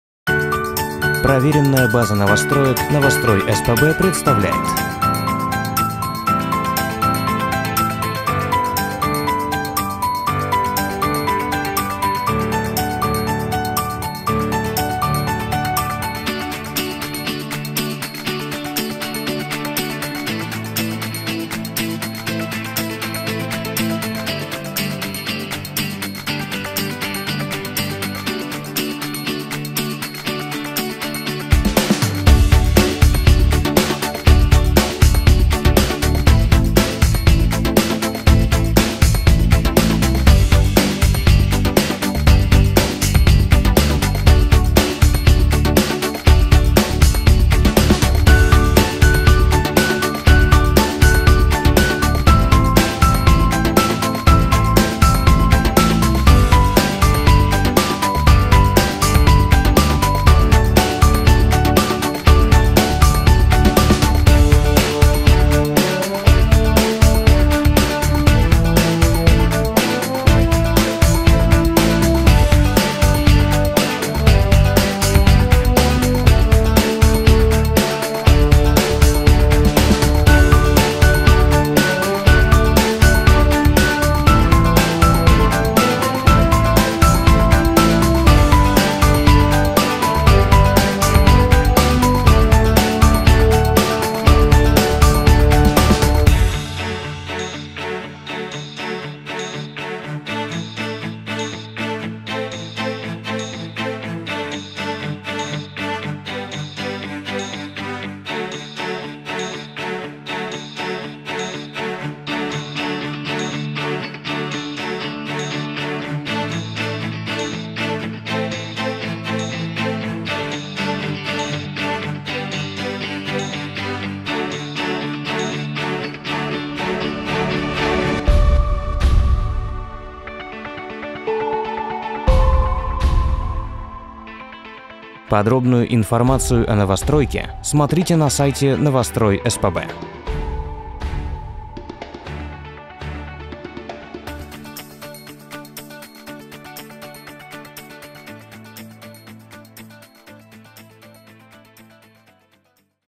ЖК «Цветной город» от застройщика «ЛСР. Недвижимость – Северо-Запад» (аэросъемка: ноябрь 2017 г.)
Подробные схемы проезда к ЖК, фото со стройки, а также строительную документацию смотрите на сайте Novostroy-SPb.ru

ЖК «Цветной город» возводится в Красногвардейском районе Санкт-Петербурга, в 2 минутах езды на транспорте от станции метро «Академическая».

Застройщик проекта — компания «ЛСР. Недвижимость – Северо-Запад».  Информацию о застройщике можно прочитать на сайте Novostroy-SPb.ru

Срок сдачи: IV кв. 2018 г.
Тип проекта: панель
Этажность: 25
Комнатность: 1, 2, 3
Площадь квартир: 30,5 – 78,5 кв. м